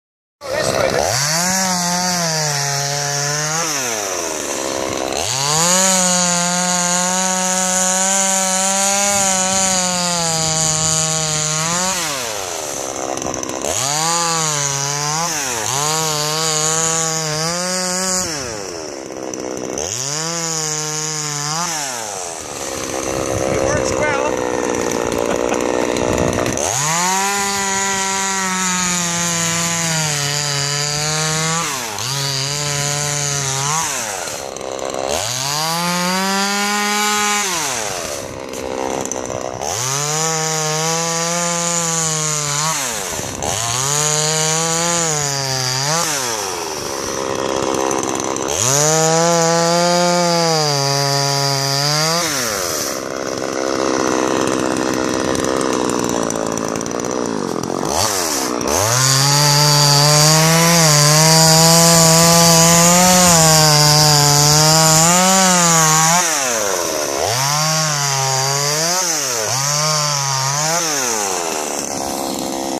RAW: Chainsaw used to clear snow after N.S storm
The snow ought to be running scared in one Nova Scotia man's driveway — he's using a chainsaw to cut through the packed snow & ice. For the complete story